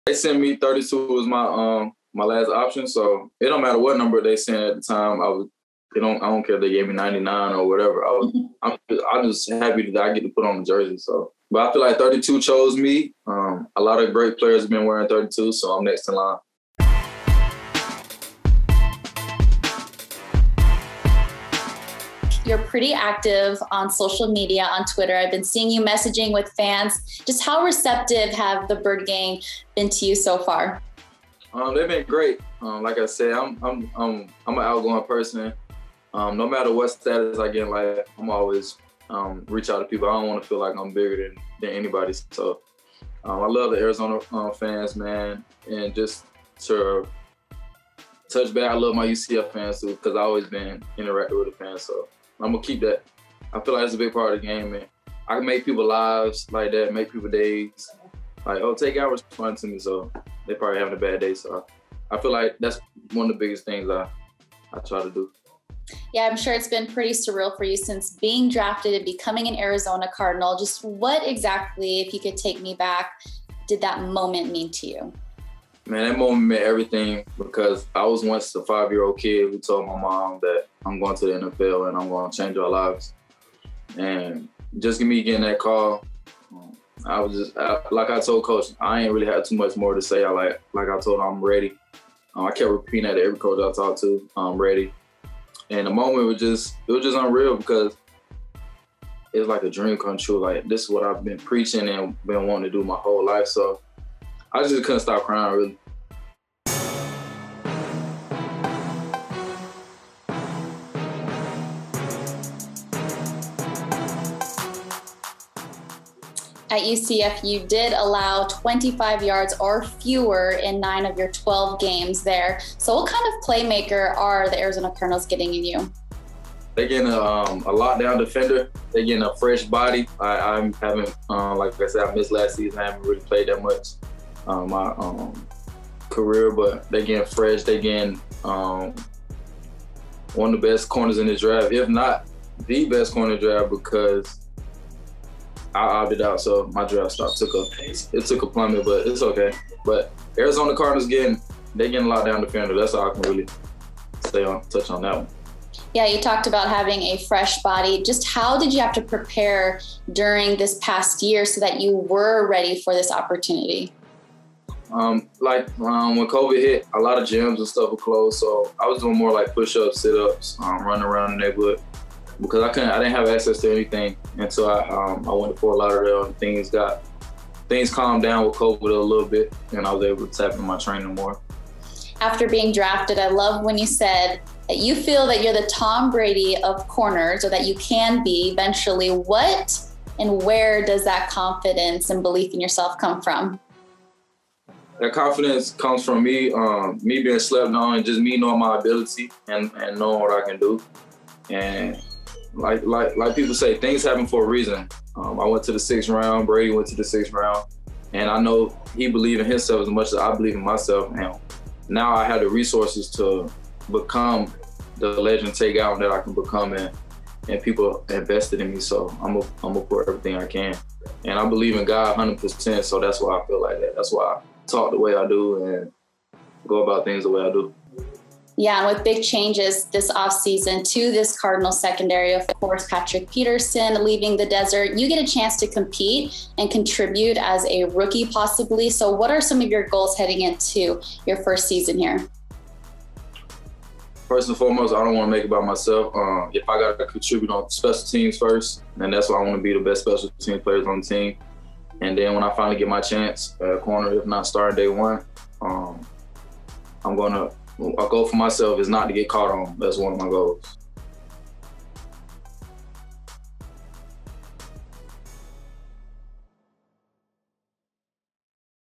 Tay Gowan Knew He Was Going to the NFL Since 5 Years Old | Arizona Cardinals
Arizona Cardinals CB Tay Gowan went OnTheFly with Insider Lisa Matthews to talk about his journey to the NFL, his confidence, wearing No. 32 and more.